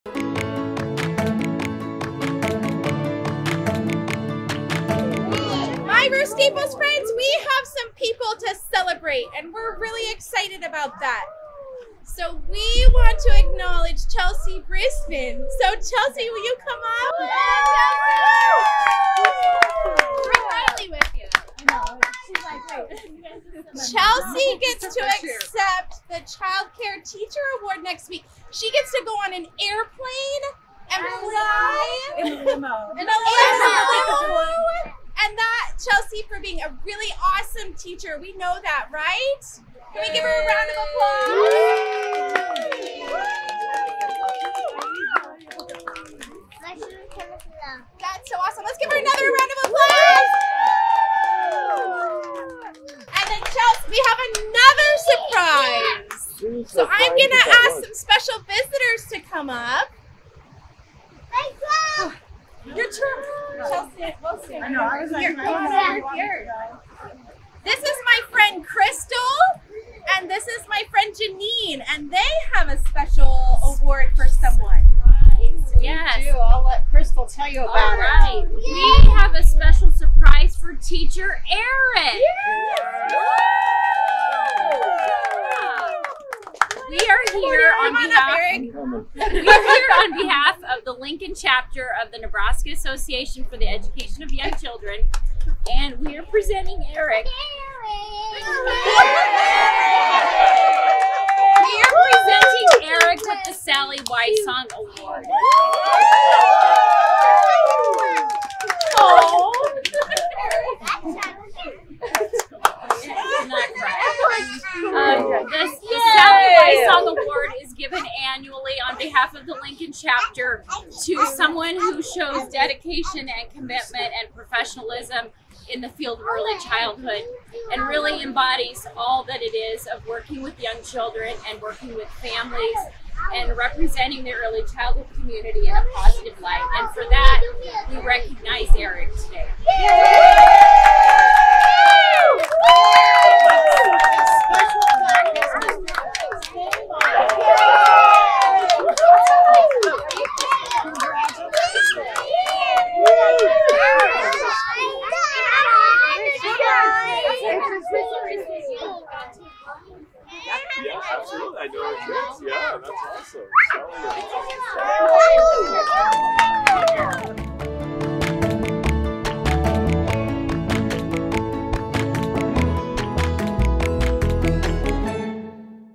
Ruth Staples CDL Honors Two of Its Teachers for Their Recognition
Awards presented to two of Ruth Staples' outstanding teachers, Chelsea Brisbin and Eric Unrau. Ruth Staples Child Development Lab; Department of Child, Youth and Family Studies (CYAF); College of Education and Human Sciences (CEHS); University of Nebraska-Lincoln (UNL)